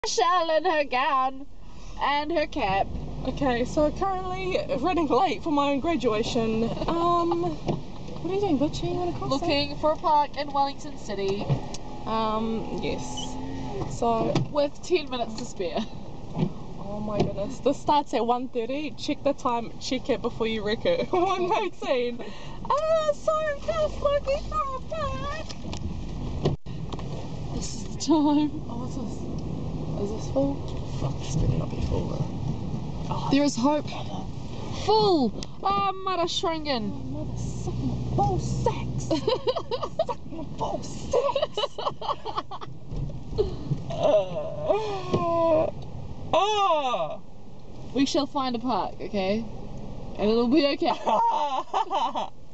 LATE FOR GRADUATION
My girl Michelle streesin out before her ceremony lol. Dont worry she made it in time.